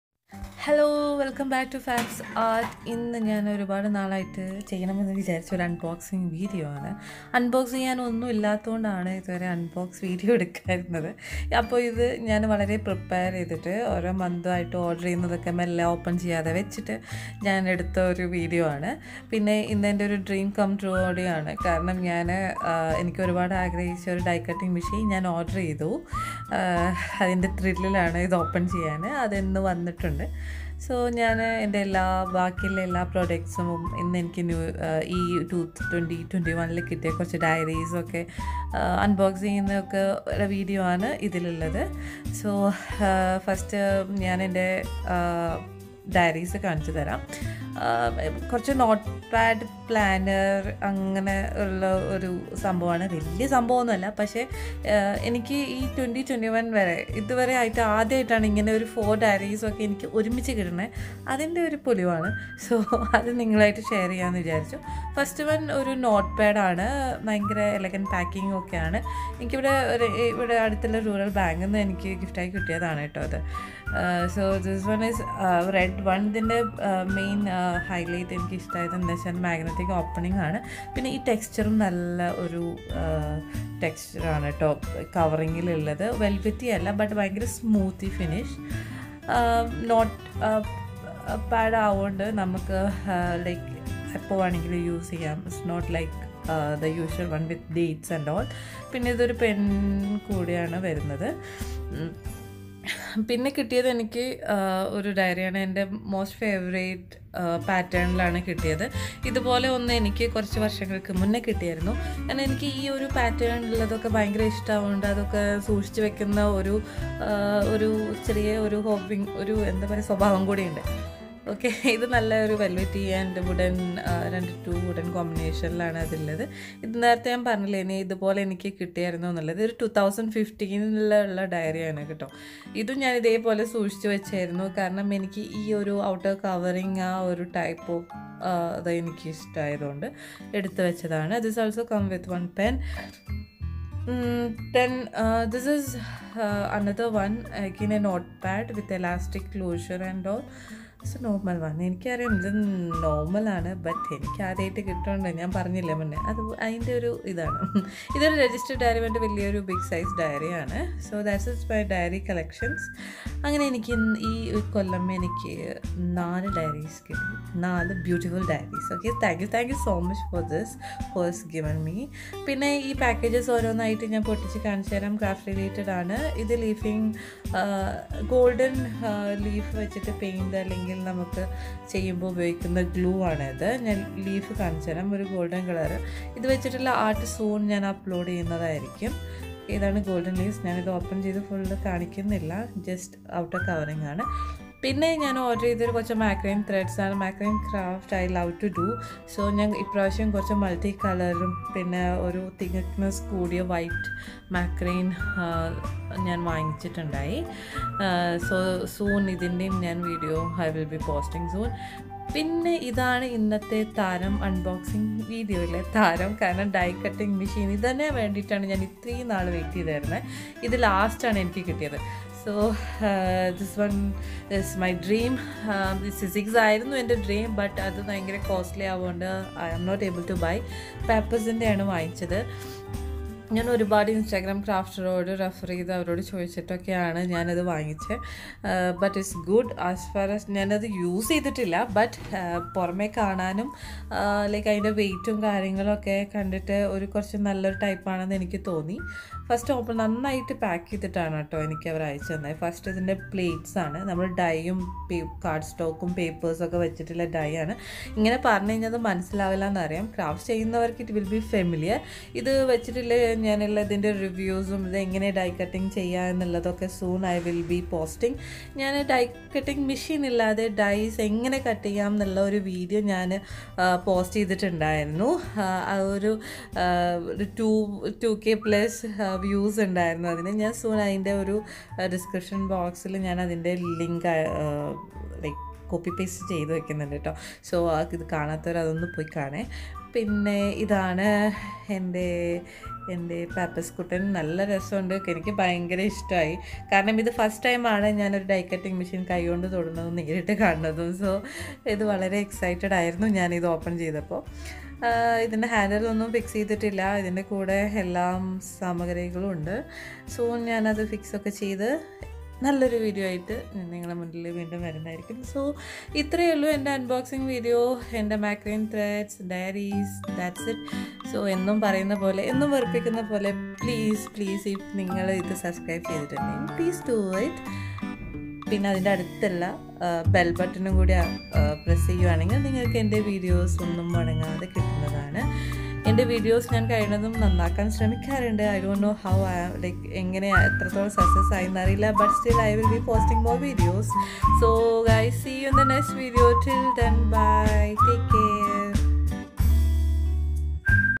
Die Cutting Machine | Unboxing Papus Die Cutting Machine | Unboxing Video | FabzsArt
Hi guyz..

Itz an unboxing video hope you all wil like it..

Here I am sharing the link of video about Cutting dies without die cutting machine.. Do check it out..
Keep supporting.
FabzsArt